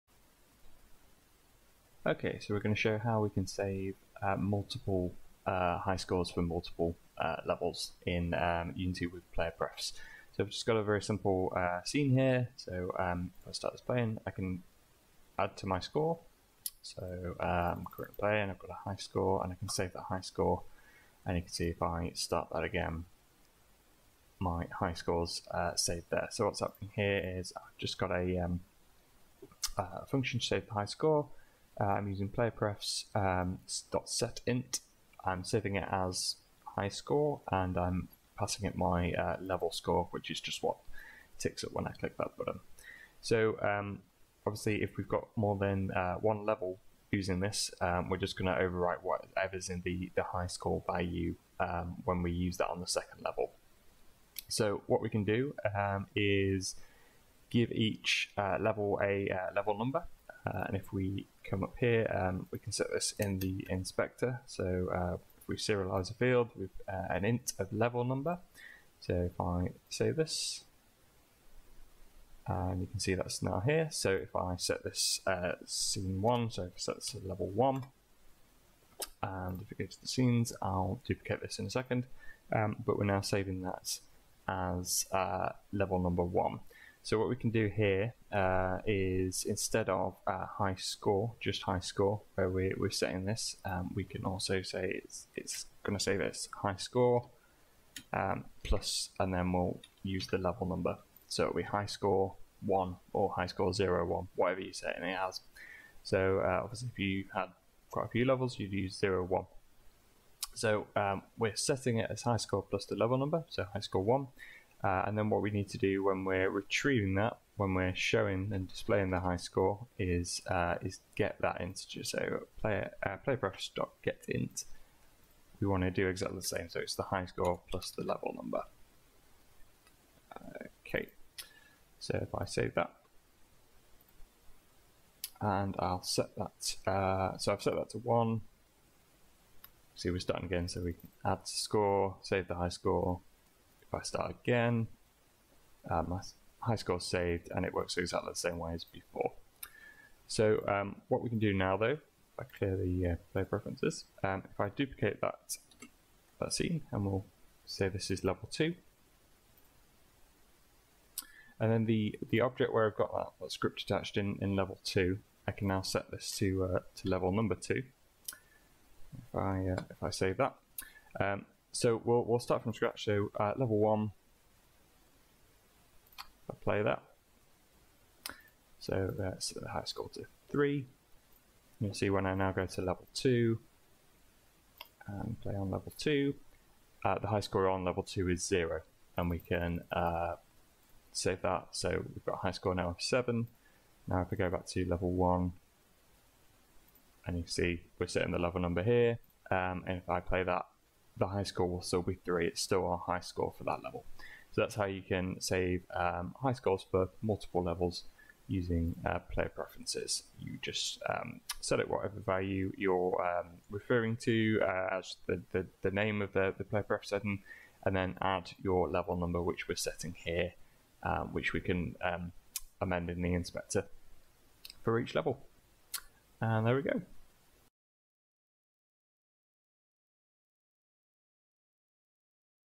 Saving Multiple Highscores for Many Levels - Unity Tutorial
An example of how you can use PlayerPrefs to save highscores for more then one level on Unity.